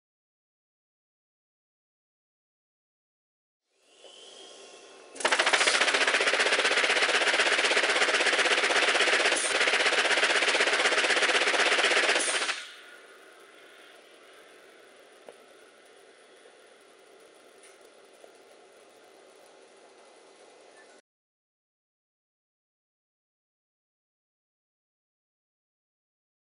Quickly Settle Product & Reduce the Angle of Repose Instantly
Roller Section Integrated Compaction Tables can be used in a roller conveyor system to quickly settle product and reduce the angle of repose instantly.